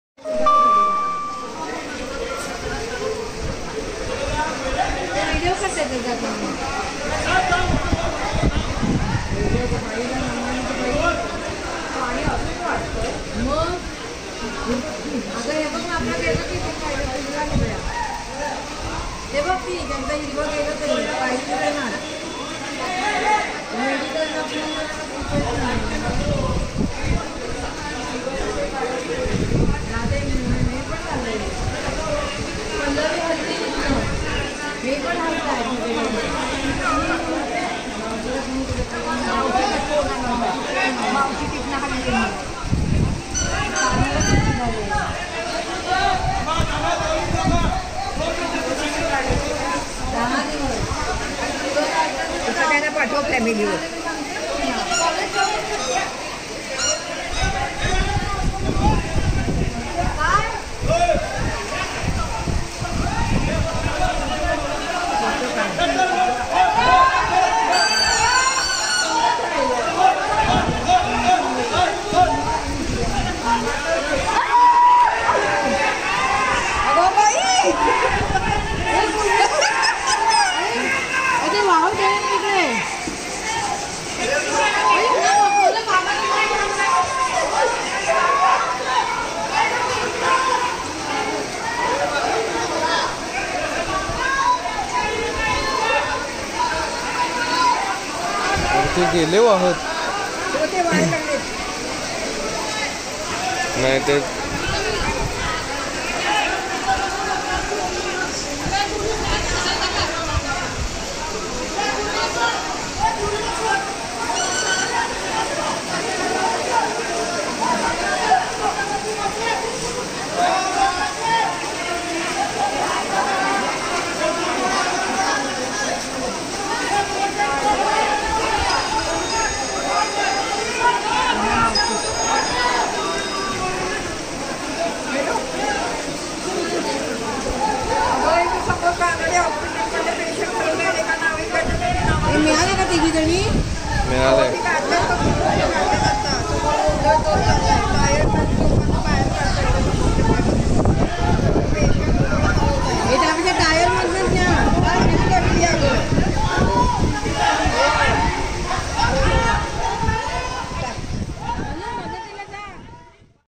कोल्हा'पूर' : रेस्क्यू करताना बोट पलटी
कोल्हापूरच्या महापुरात व्हिनस कॉर्नर येथे रेस्क्यू करताना बोट पलटल्याची घटना घडली. तीन महिलांना अडकलेल्या ठिकाणाहून बाहेर काढताना अचानक ही बोट पलटी झाली. त्यांना वेळीच बचाव पथकाने मदत करत सुरक्षितस्थळी हलवले.

कोल्हापूरात पुराच्या पाण्यामुळे अनेकजण घरांमध्ये अडकले आहेत. त्यांना तेथून बाहेर काढण्यासाठी 700 ते 800 जणांची टीम कार्यरत आहे. बचावकार्यासाठी एनडीआरफचे पथक सुद्धा तैनात करण्यात आले आहे. हेच बचावकार्य सुरू असताना ही घटना घडली. काल पासून दिवस रात्र हे बचाव कार्य सुरू आहे. 20 हजारांहून अधिकांचे सुरक्षित ठिकाणी स्थलांतर करण्यात आले आहे. या दरम्यानच ही एक घटना घडली. तिन्ही महिला आता सुखरूप असून सुरक्षितस्थळी त्यांना हलविण्यात आले आहे.